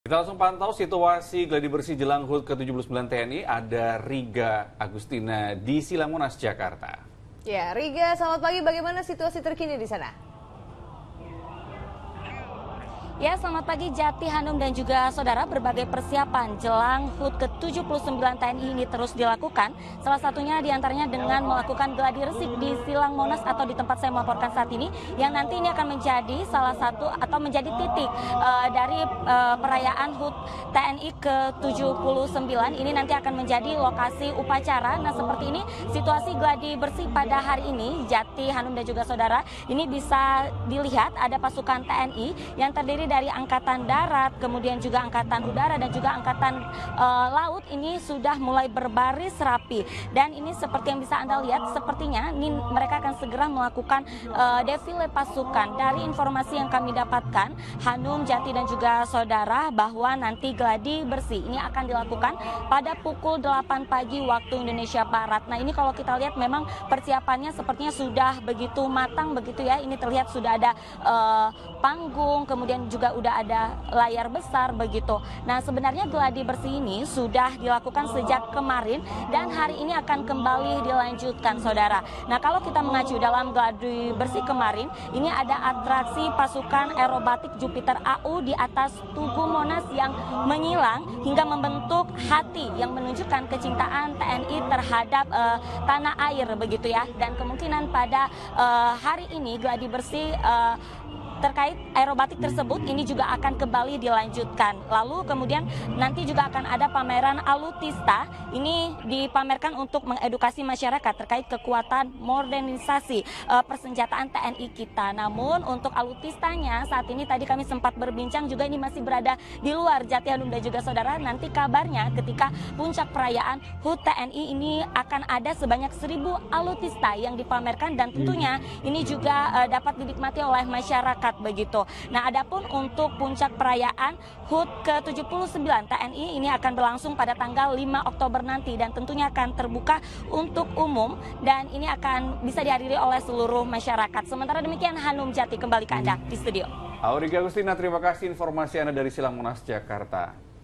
Gladi Bersih Jelang Upacara HUT TNI ke-79 di Monas | Liputan 6
kita pantau situasi terkini gladi bersih jelang hut ke 79 tni . sudah ada rekan kami auriga agustina dan marcellinus di silang monas jakarta.